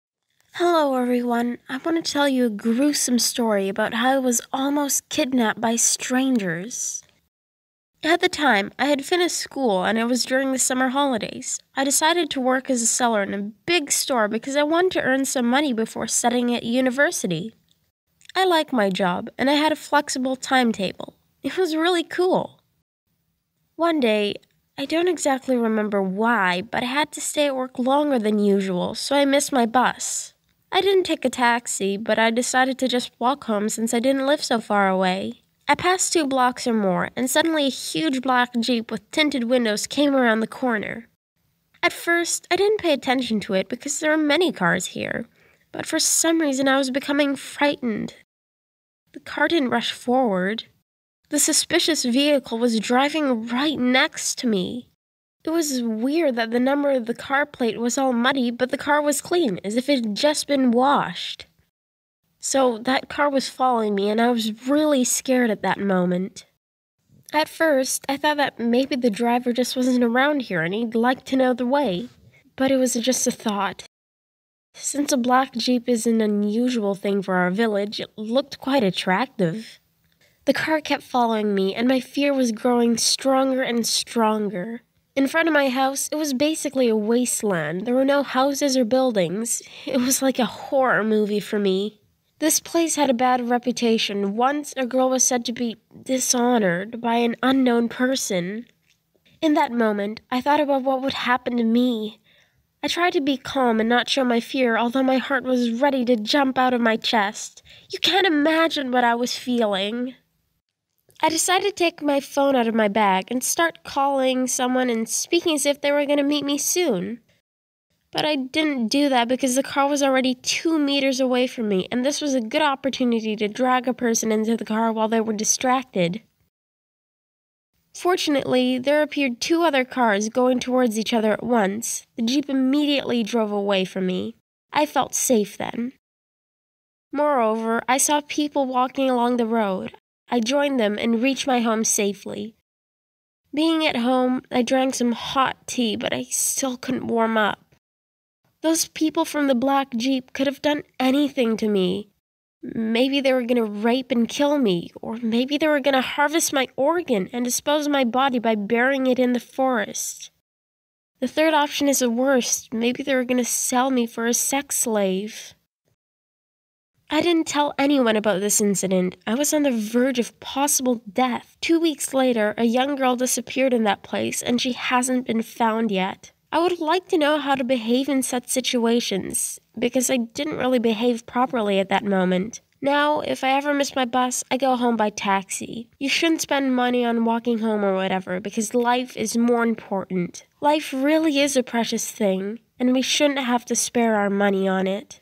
I was nearly stolen | It was scary
One day I don’t remember exactly why but I had to stay at work longer than usually so I missed my bus. I didn’t take a taxi but decided to walk home as I lived not so far. I passed two blocks or more and suddenly a huge black jeep with tinted windows came around the corner. At first, I didn’t pay attention to it because there are usually many cars here. But for some reason I was becoming frightened. The car didn’t rush forward; a suspicious vehicle was driving next to me. It was interesting that number plate of the car was muddy but the car was clean as if it was just after washing. So that car was following me and I was really scared at that moment.